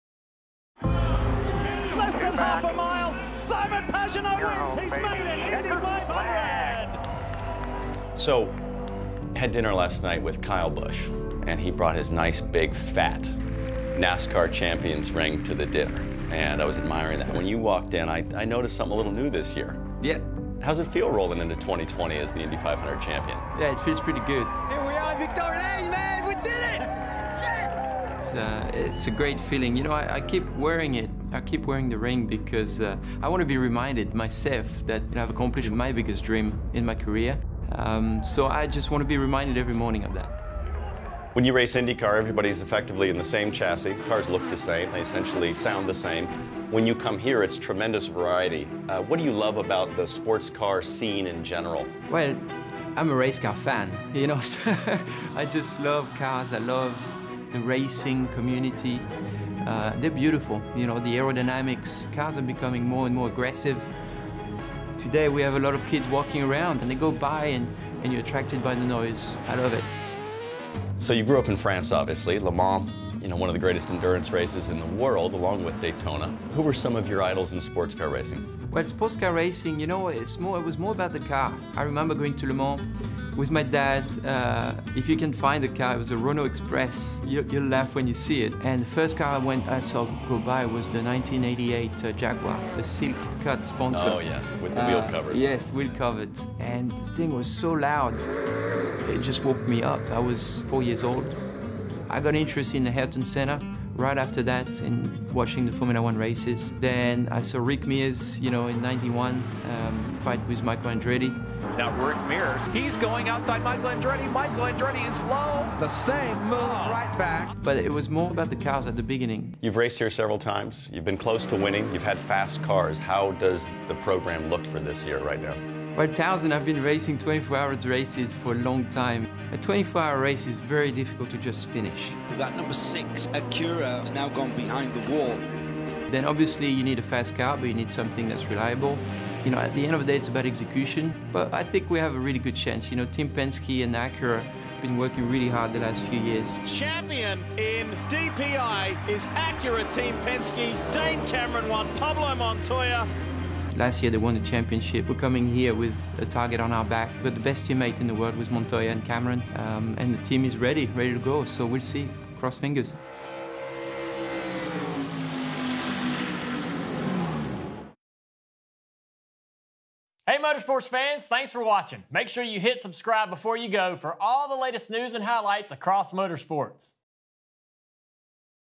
Simon Pagenaud on repeating at IMSA Rolex 24, his love for sports car scene | Motorsports on NBC
Indy 500 winner Simon Pagenaud explains what he loves about the sports car scene, reveals the first sports car he ever saw racing and looks ahead to this year's Rolex 24.

Simon Pagenaud on repeating at Rolex 24, his love for sports car scene | Motorsports on NBC